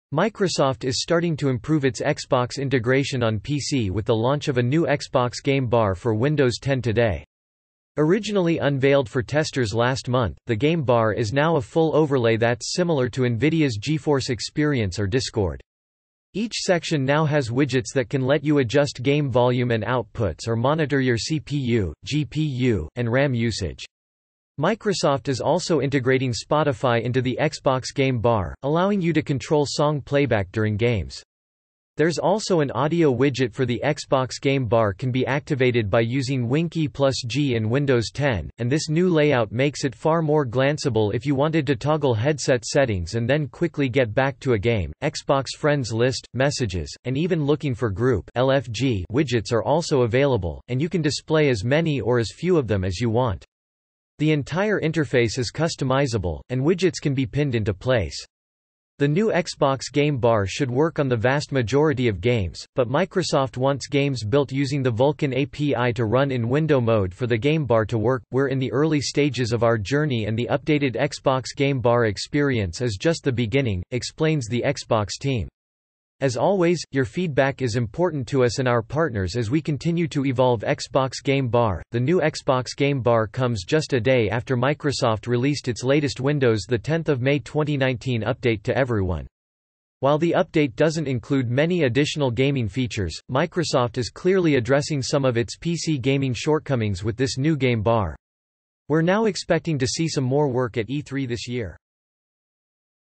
Microsoft’s new Xbox Game Bar launches for Windows 10 as a useful overlay with widgets
Microsoft is starting to improve its Xbox integration on PC with the launch of a new Xbox Game Bar for Windows 10 today. Originally unveiled for testers last month, the Game Bar is now a full overlay that’s similar to Nvidia’s GeForce Experience or Discord. Each section now has widgets that can let you adjust game volume and outputs or monitor your CPU, GPU, and RAM usage.